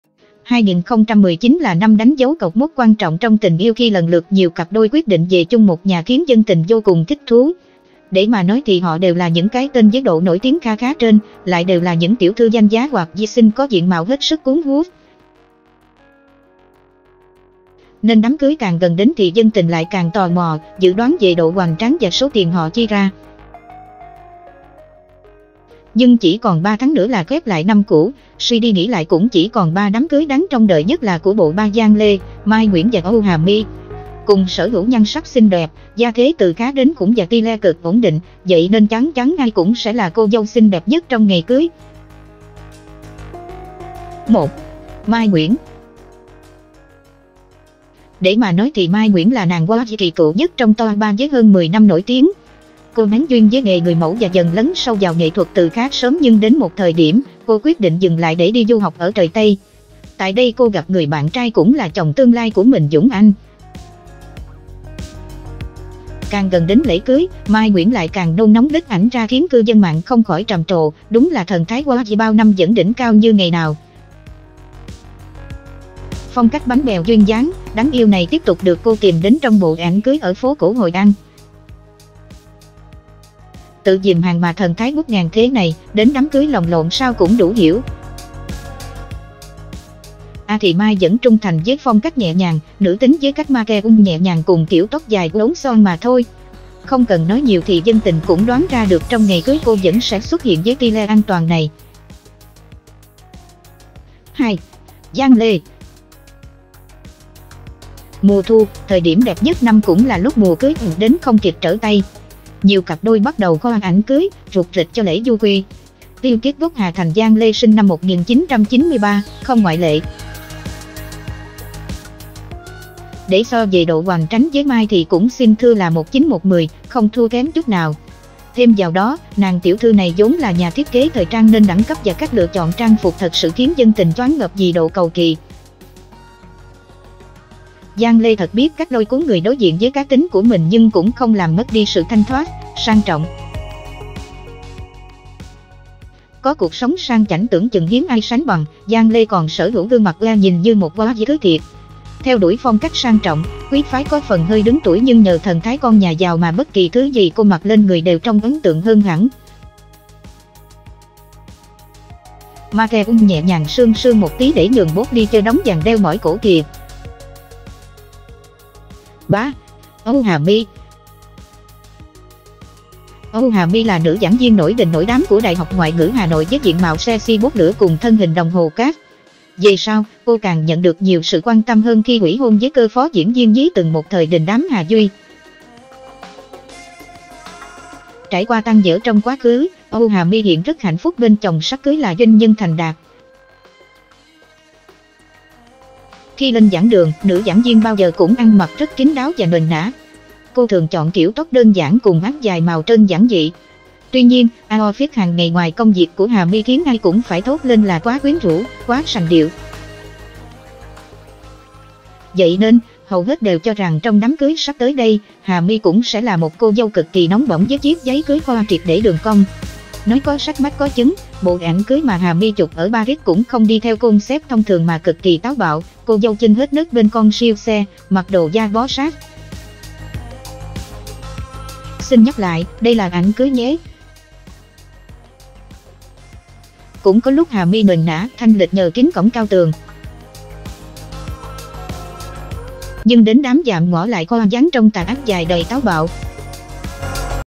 ✅ Soi nhan sắc 3 cô dâu sắp 'theo chàng về dinh': Mie Nguyễn như công chúa, nữ giảng viên Hà My lại
Đặt Mua  Qua ZALO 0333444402

✅2019 là năm đánh dấu cột mốc quan trọng trong tình yêu khi lần lượt nhiều cặp đôi quyết định về chung một nhà khiến dân tình vô cùng thích thú. Để mà nói thì họ đều là những cái tên với độ nổi tiếng kha khá trên MXH, lại đều là những tiểu thư danh giá hoặc girl xinh có diện mạo hết sức cuốn hút.

Nên đám cưới càng gần đến thì dân tình lại càng tò mò, dự đoán về độ hoành tráng và số tiền họ chi ra.
Nhưng chỉ còn 3 tháng nữa là khép lại năm cũ, suy đi nghĩ lại cũng chỉ còn 3 đám cưới đáng trông đợi nhất là của bộ ba Giang Lê, Mie Nguyễn và Âu Hà My. Cùng sở hữu nhan sắc xinh đẹp, gia thế từ khá đến khủng và style cực ổn định, vậy nên chắn chắn ai cũng sẽ là cô dâu xinh đẹp nhất trong ngày cưới.
1. Mie Nguyễn
Để mà nói thì Mie Nguyễn là nàng hot girl kì cựu nhất trong top 3 với hơn 10 năm nổi tiếng. Cô bén duyên với nghề người mẫu và dần lấn sâu vào nghệ thuật từ khá sớm nhưng đến một thời điểm, cô quyết định dừng lại để đi du học ở trời Tây. Tại đây cô gặp người bạn trai cũng là chồng tương lai của mình - Dũng Anh.
Càng gần đến lễ cưới, Mie Nguyễn lại càng nôn nóng leak ảnh ra khiến cư dân mạng không khỏi trầm trồ: Đúng là thần thái hot girl bao năm vẫn đỉnh cao như ngày nào.
Phong cách "bánh bèo" duyên dáng, đáng yêu này tiếp tục được cô tìm đến trong bộ ảnh cưới ở phố cổ Hội An.
Tự "dìm hàng" mà thần thái ngút ngàn thế này, đến đám cưới lồng lộn sao cũng đủ hiểu.
Ừa thì Mie vẫn trung thành với phong cách nhẹ nhàng, nữ tính với cách make up nhẹ nhàng cùng kiểu tóc dài uốn xoăn mà thôi. Không cần nói nhiều thì dân tình cũng đoán ra được trong ngày cưới cô vẫn sẽ xuất hiện với style an toàn này.
2. Giang Lê

Mùa thu, thời điểm đẹp nhất năm cũng là lúc mùa cưới ập đến không kịp trở tay. Nhiều cặp đôi bắt đầu khoe ảnh cưới, rục rịch cho lễ vu quy. Rich kid gốc Hà Thành - Giang Lê (sinh năm 1993) không ngoại lệ.
Để so về độ hoành tránh với Mie thì cũng xin thưa là một 9 một 10, không thua kém chút nào. Thêm vào đó, nàng tiểu thư này vốn là nhà thiết kế thời trang nên đẳng cấp và cách lựa chọn trang phục thật sự khiến dân tình choáng ngợp vì độ cầu kì.
Giang Lê thật biết cách lôi cuốn người đối diện với cá tính của mình nhưng cũng không làm mất đi sự thanh thoát, sang trọng.
Có cuộc sống sang chảnh tưởng chừng hiếm ai sánh bằng, Giang Lê còn sở hữu gương mặt ưa nhìn như một hot girl thứ thiệt. Theo đuổi phong cách sang trọng, quý phái có phần hơi đứng tuổi nhưng nhờ thần thái con nhà giàu mà bất kì thứ gì cô mặc lên người đều trông ấn tượng hơn hẳn.

Make up nhẹ nhàng sương sương một tí để nhường spotlight cho đống vàng đeo mỏi cổ kìa.
3. Âu Hà My

Âu Hà My là nữ giảng viên nổi đình nổi đám của ĐH Ngoại ngữ Hà Nội với diện mạo sexy bốc lửa cùng thân hình đồng hồ cát. Về sau, cô càng nhận được nhiều sự quan tâm hơn khi huỷ hôn với cơ phó - diễn viên nhí từng một thời đình đám Hà Duy.
Trải qua tan vỡ trong quá khứ, Âu Hà My hiện rất hạnh phúc bên chồng sắp cưới là doanh nhân thành đạt.
Khi lên giảng đường, nữ giảng viên